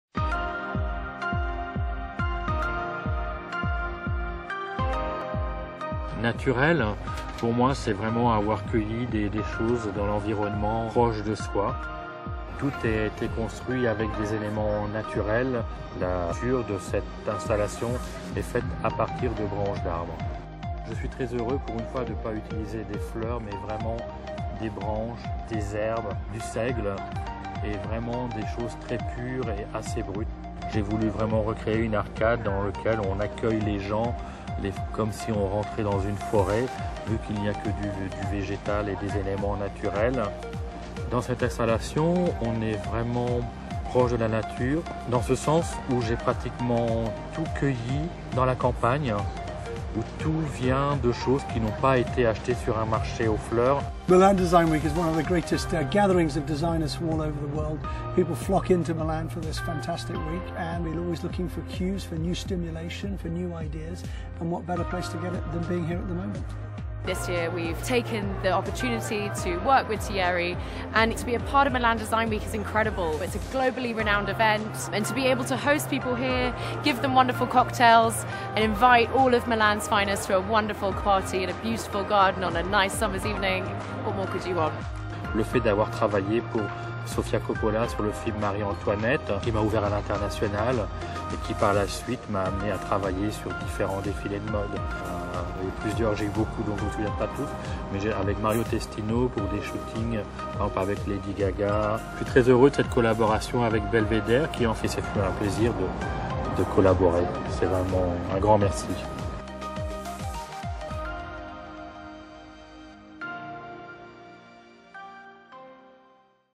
Thierry Boutemy unveils installation at Belvedere Garden during Milan Design Week
Belvedere vodka, the world’s original luxury vodka, has partnered with prominent floral designer thierry boutemy to create an immersive nature experience, unveiled at the belvedere garden during milan design week.

Boutemy created a beautiful installation made only from the raw materials in Belvedere Vodka and its spring/summer Spritz cocktail collection – superior Polish Dankowskie rye, fresh herbs, citrus and cucumber. The experience is a celebration of the brand’s commitment to natural ingredients and simple cocktails.